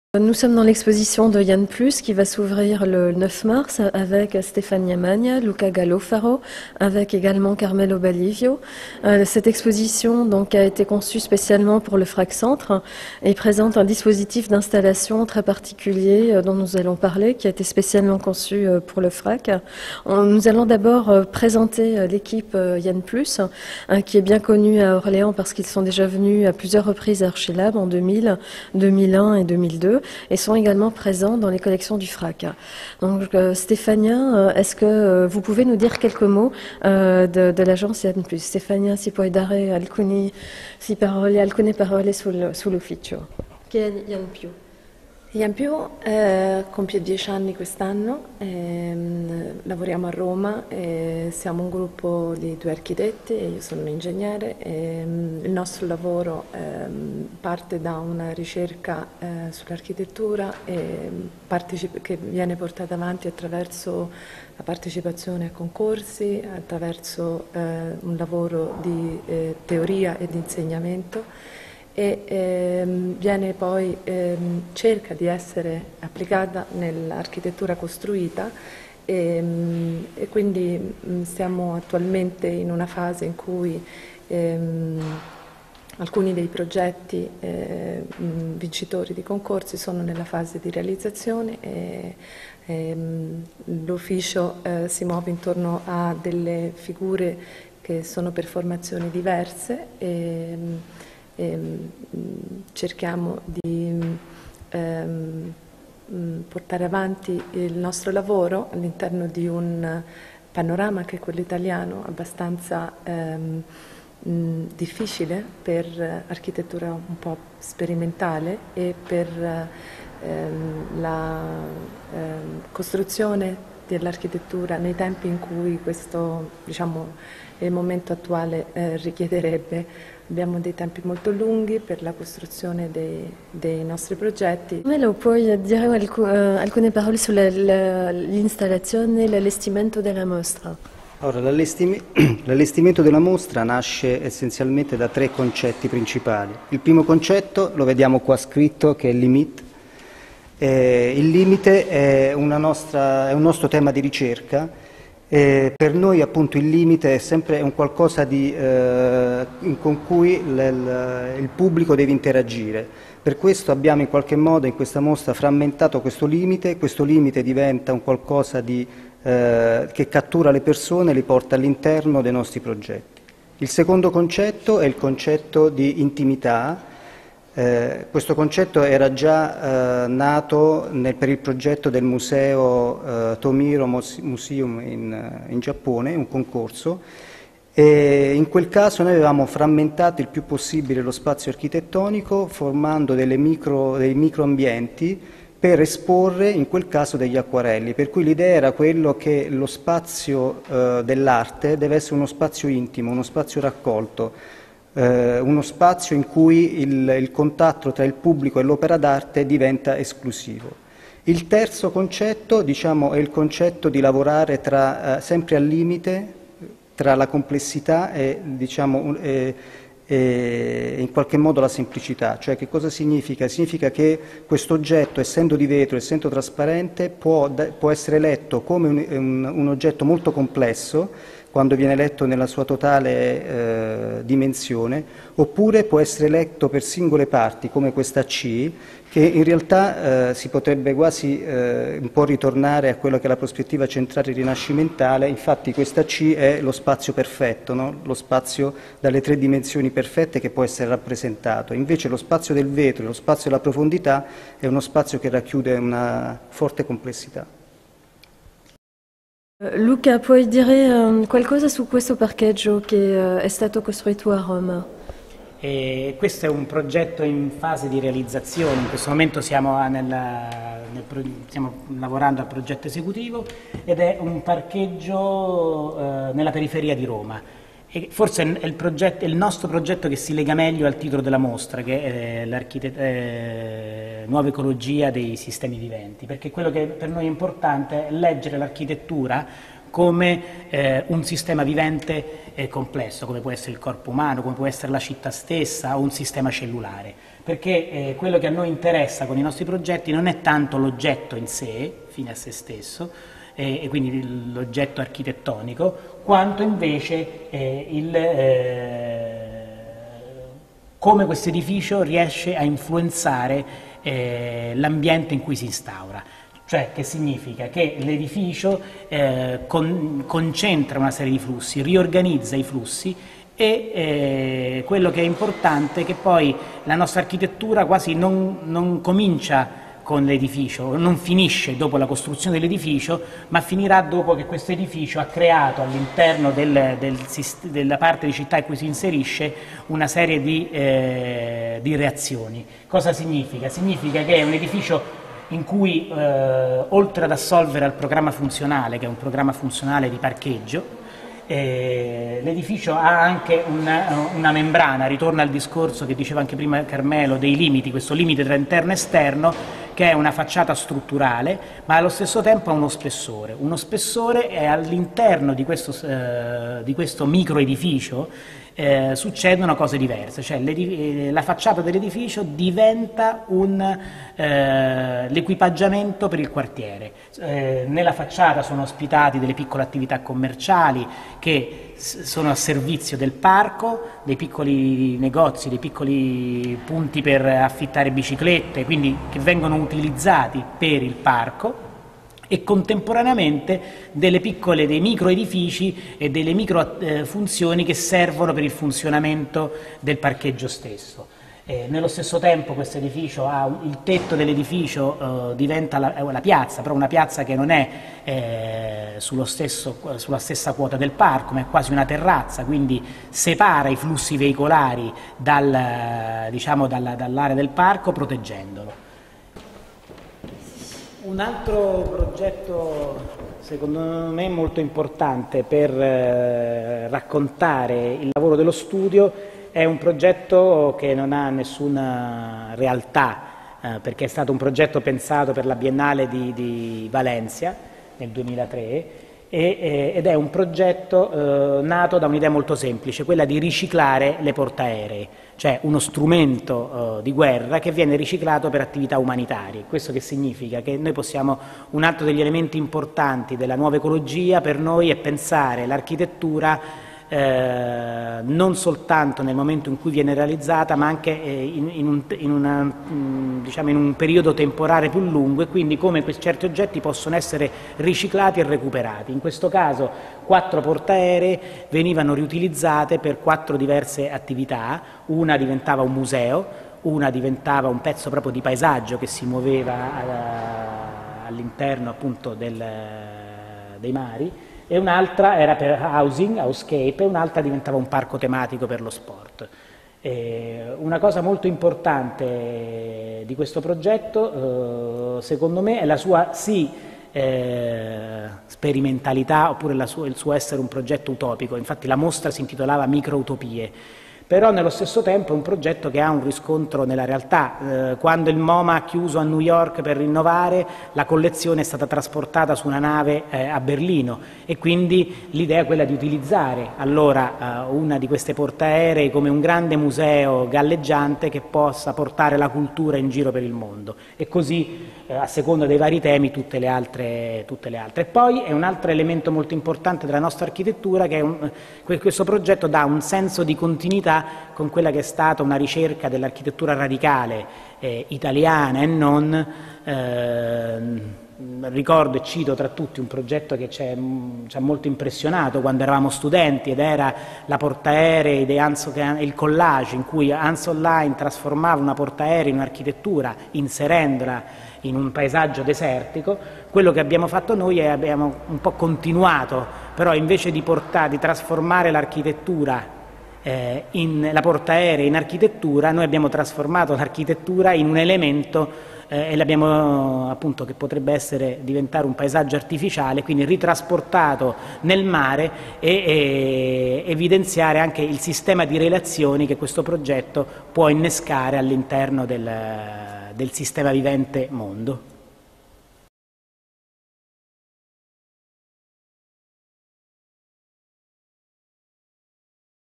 [IT/FR] Entretien avec IaN+ | Frac Centre-Val de Loire
IaN+ est une agence pluridisciplinaire, entre recherche théorique et pratique architecturale, créée à Rome en 1997. L’agence se structure autour de trois personnes : Carmelo Baglivo et Luca Galofaro sur l'architecture et Stefania Manna sur l’ingénierie. Ils prennent la parole dans le cadre de l'exposition « Ian+ - Nouvelle écologie des systèmes vivants » organisée du 10 mars au 6 mai 2007 par le Frac Centre-Val de Loire.